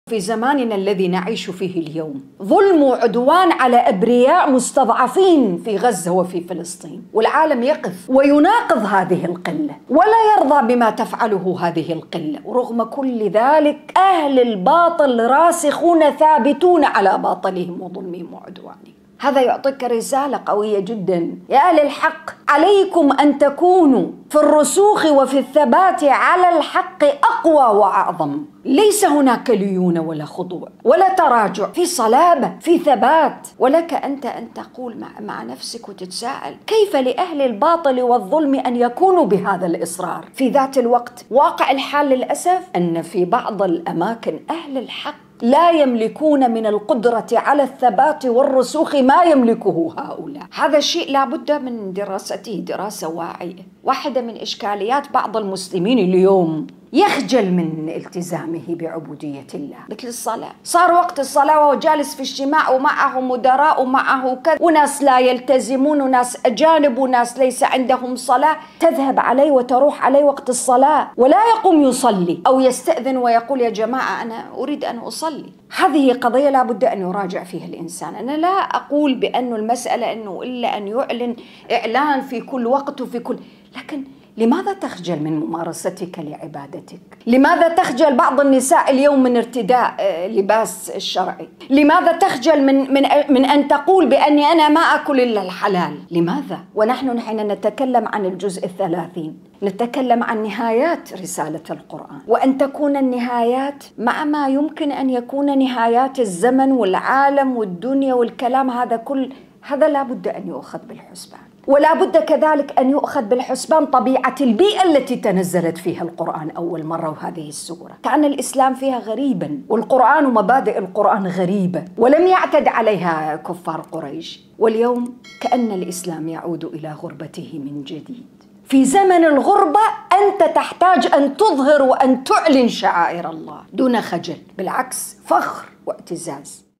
مقطع قصير من تدبر سورة الكافرون 2
عجبًا لثبات أهل الباطل على باطلهم؛ الذي يعلمون!!!
وخجل أهل الحق من حقّ به يؤمنون
سيقف الجمع أمام الحقّ، ويُسئلون

الحسابات الرسمية لـ أ.د. رقية طه العلواني

 / drruqaiaalal.  .

Transcript